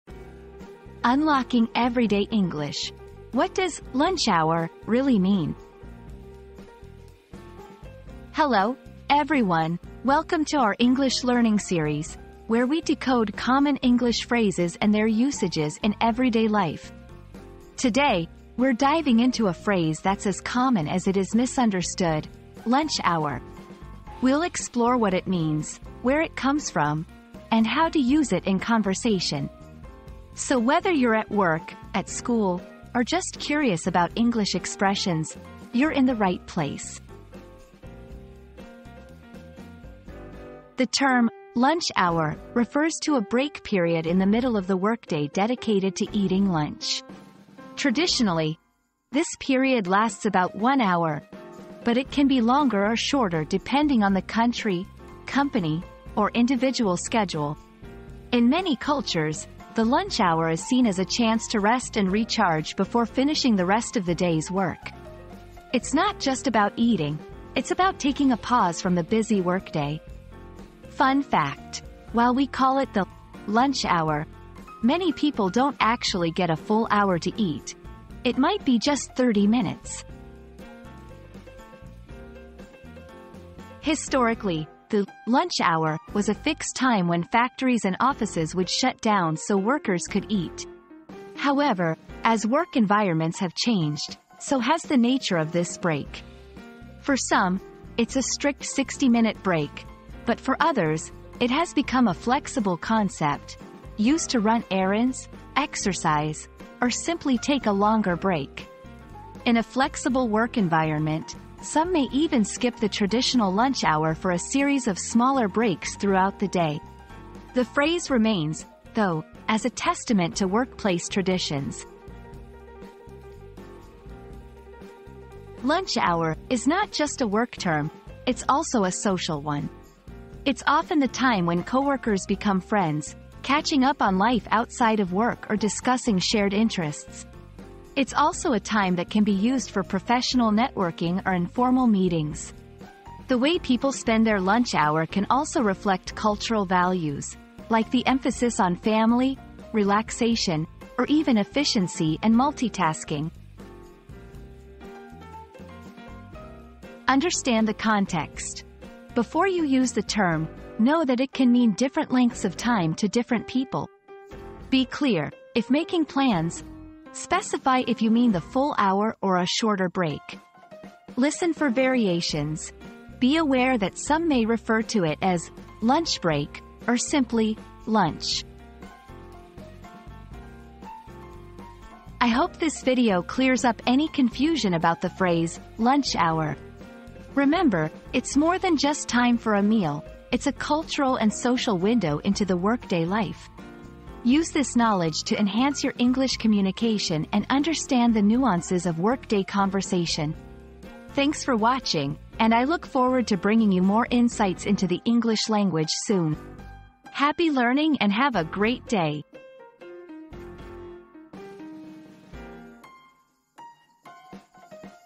Unlocking Everyday English: What Does "Lunch Hour" Really Mean?
Unlocking Everyday English: What Does 'Lunch Hour' Really Mean? • Discover the true meaning behind 'Lunch Hour' and unravel its significance in everyday English conversations. Join us as we delve into the origins and cultural context of this commonly used phrase, shedding light on its hidden nuances and practical applications.

00:00 • Introduction - Unlocking Everyday English: What Does "Lunch Hour" Really Mean?
00:40 • Understanding "Lunch Hour"
01:23 • The Evolution of "Lunch Hour"
02:05 • "Lunch Hour" in Social Context
02:36 • Tips for Using "Lunch Hour" in English